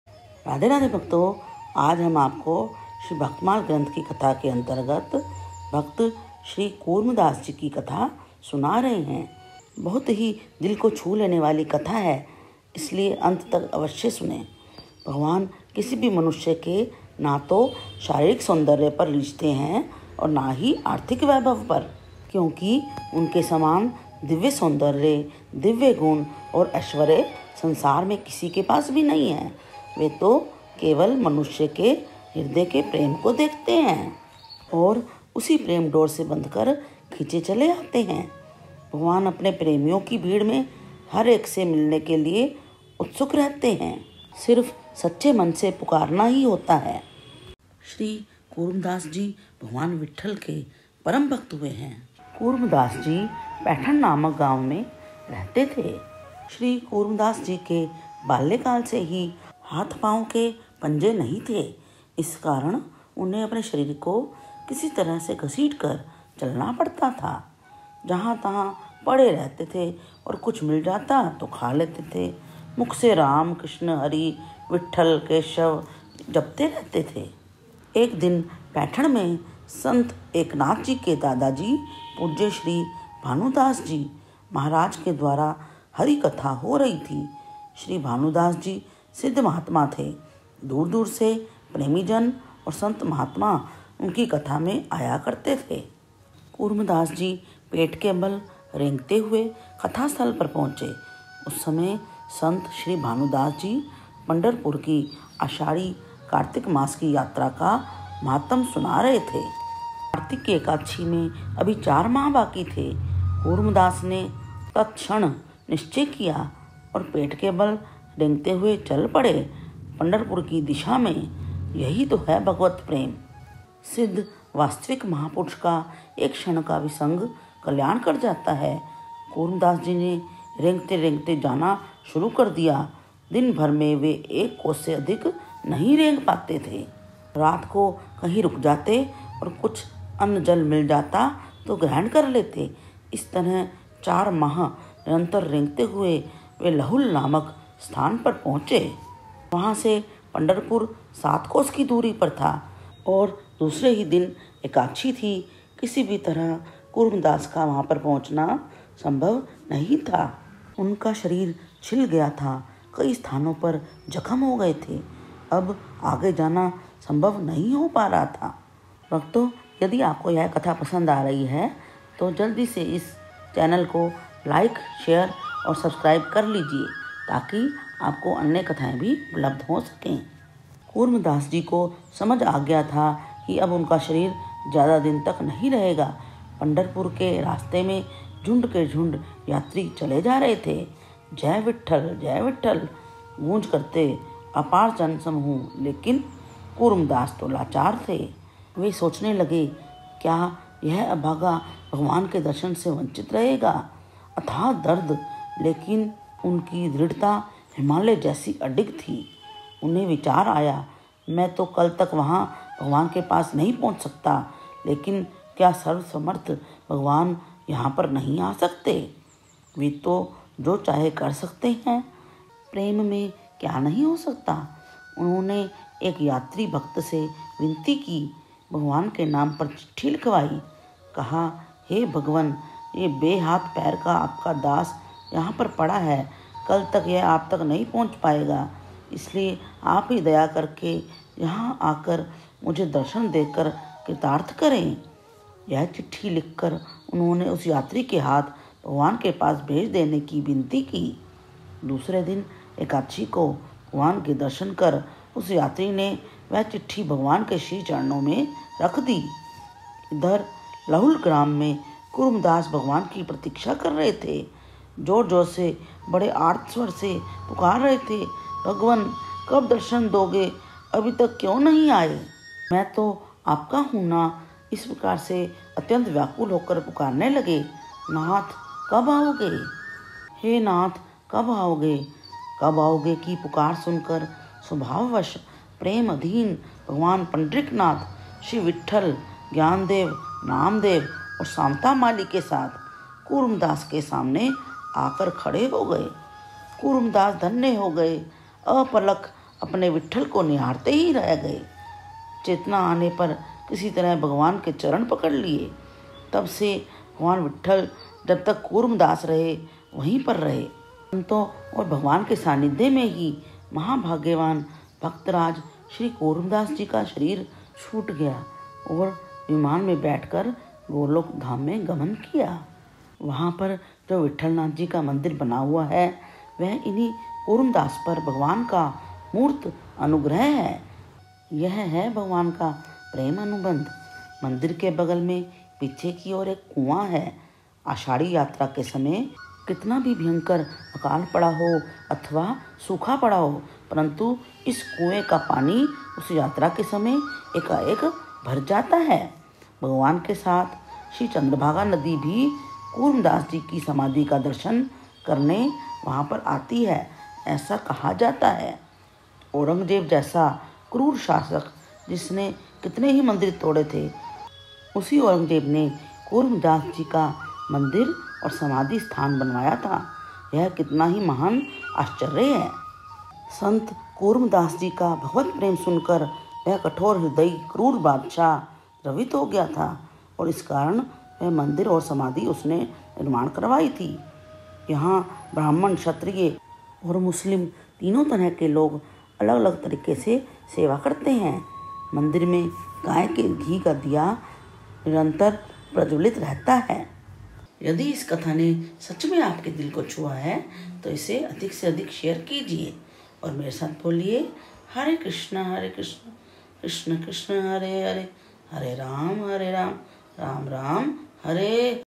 bhaktmal Katha 43#bhakt कुर्मदास पेट के बल रेंगते हुए चल पड़े पंडरपुर की दिशा में परंतु रास्ते में
# bhaktmal katha# भक्तमाल कथा# भक्त bhakt # bhagvan k bhakt # भगवान के भक्त
bhakt charitr # भक्त चरित्र